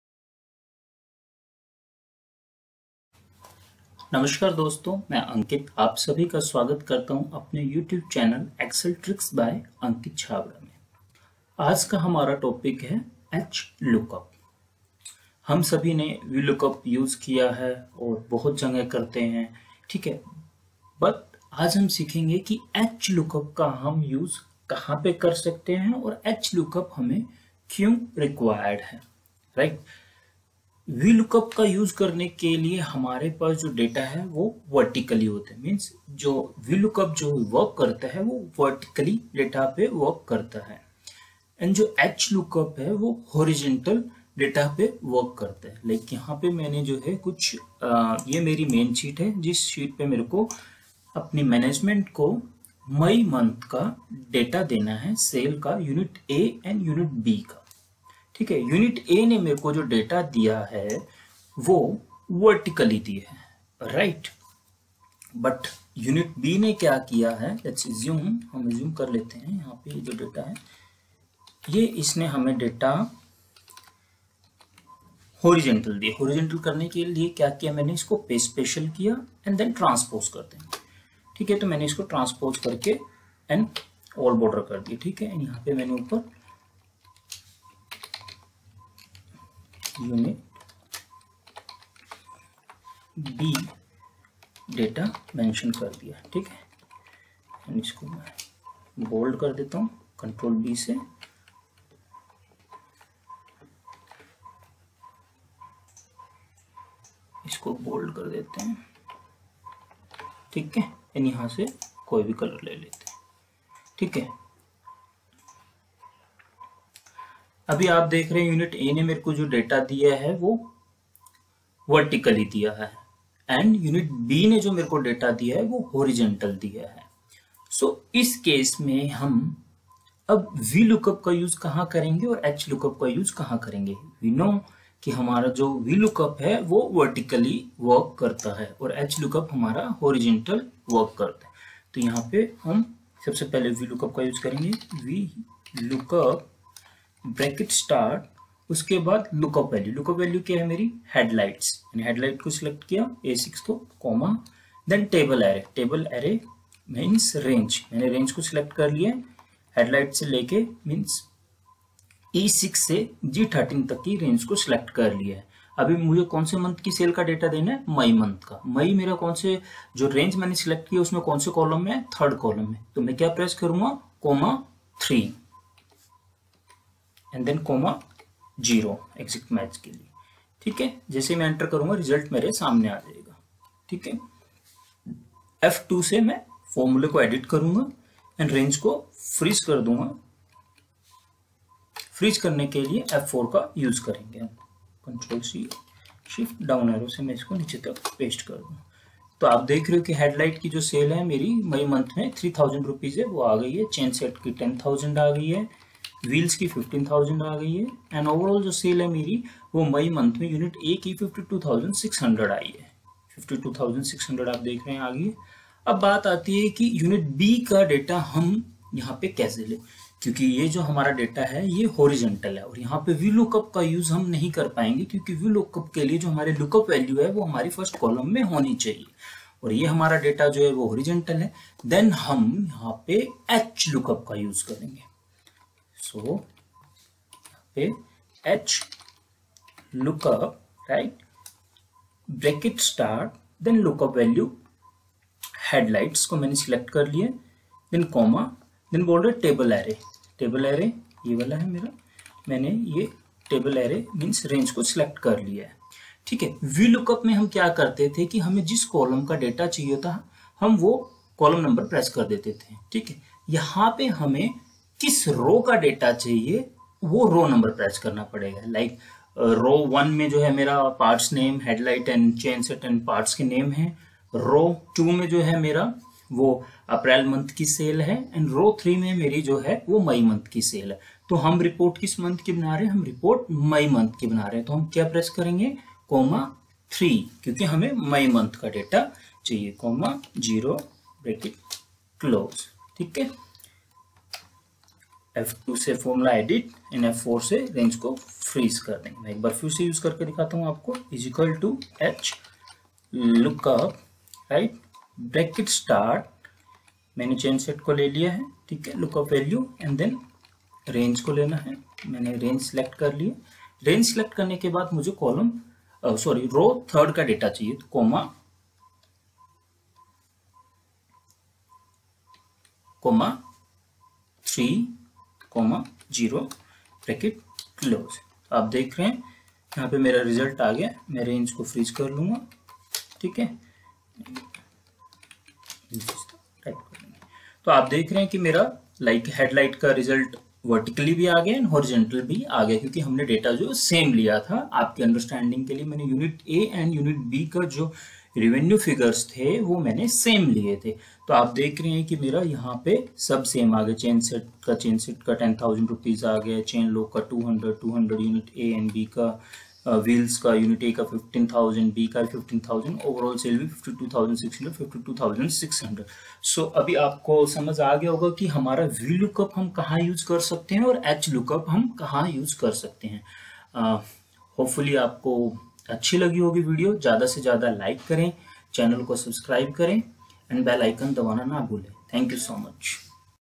Learning about Hlookup, Why Hlookup is required
In this topic, we will learn about Hlookup function in Excel. what is the major difference in Vlookup and Hlookup and where we can use vlookup and hlookup and why hlookup is required. Lets Learn and grow together.